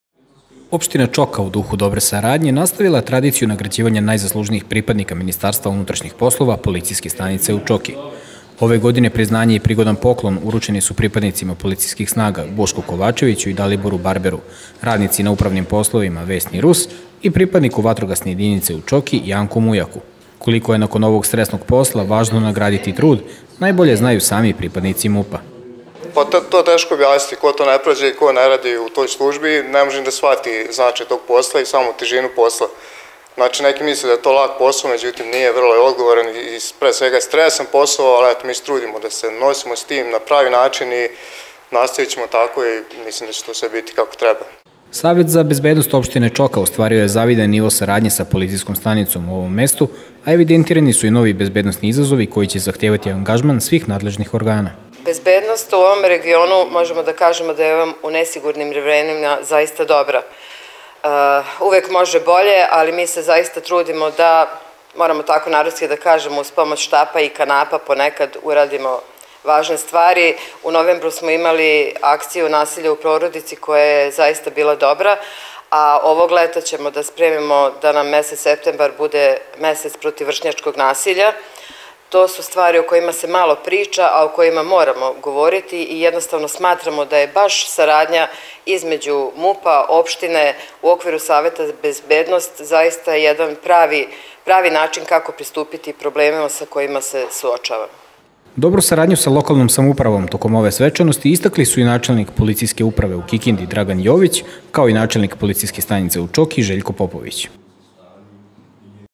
Čoka nagradila najzaslužnije pripadnike MUP-a
Opština Čoka nagradila je istaknute pripadnike MUP-a i time potvrdila dobru saradnju sa bezbednosnim organima.